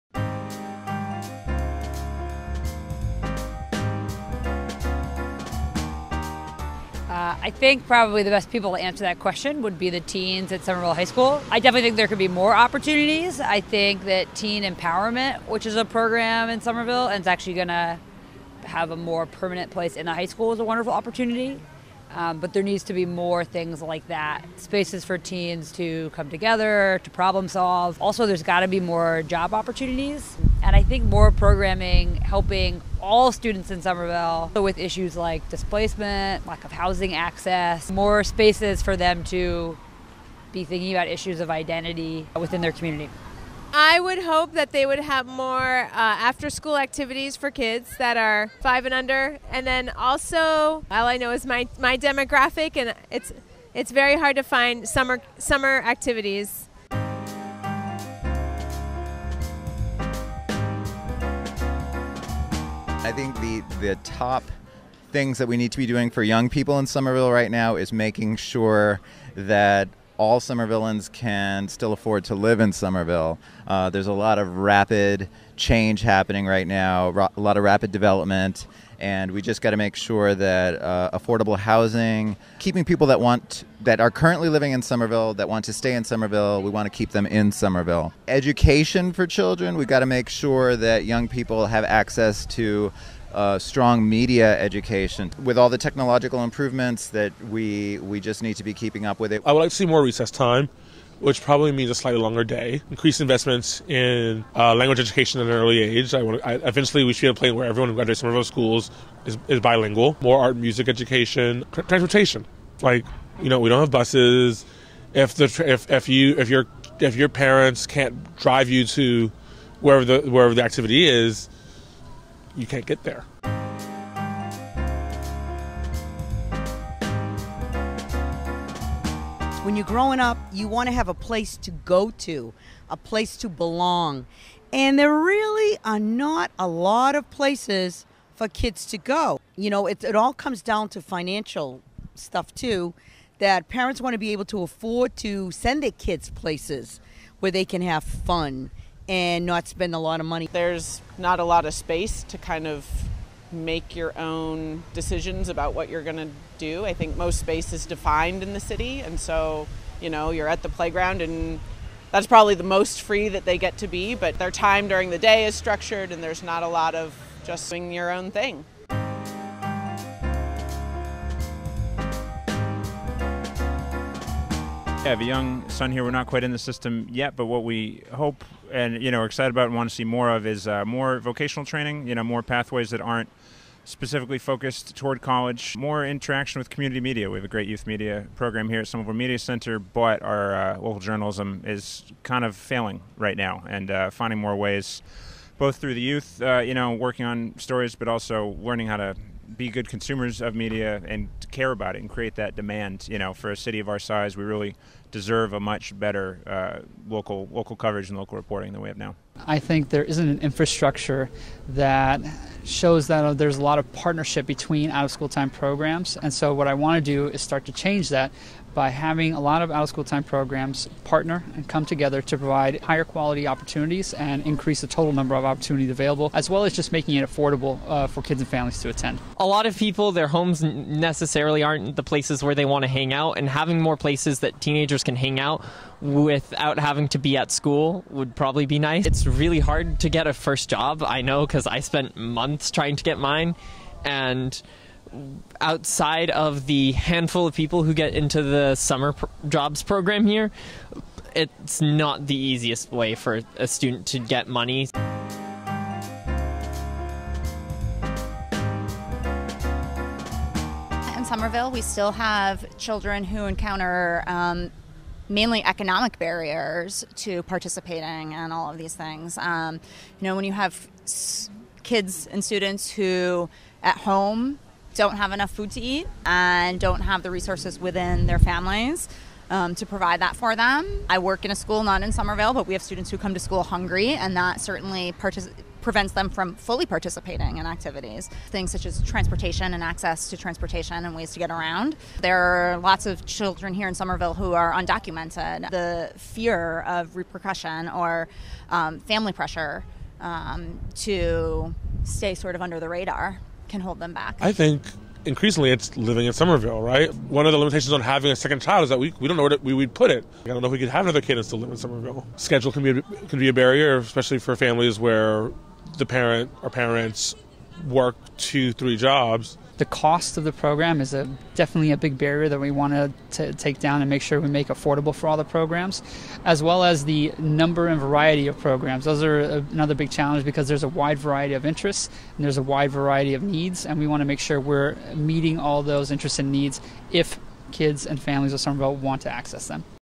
Somerville Learning 2030: Improving Education and Learning in Somerville
We had young people interview folks on the street about their perspectives on learning in Somerville--do all young people have equitable access to learning opportunities in the City? Do certain age groups or other demographics need special attention to make sure their needs are met? How are we doing on our commitment to provide high quality education--both inside and out of school--to all children in our city? Teachers, community members, students and city officials weigh in in this mini-focus group video for Somerville Learning 2030.

Produced by teen interns Ben Bortner, Anya Pattinson, Carolina Carneiro, and Deha Ahmed. Funding for this project made possible by Somerpromise and the City of Somerville's Somerville Learning 2030 project.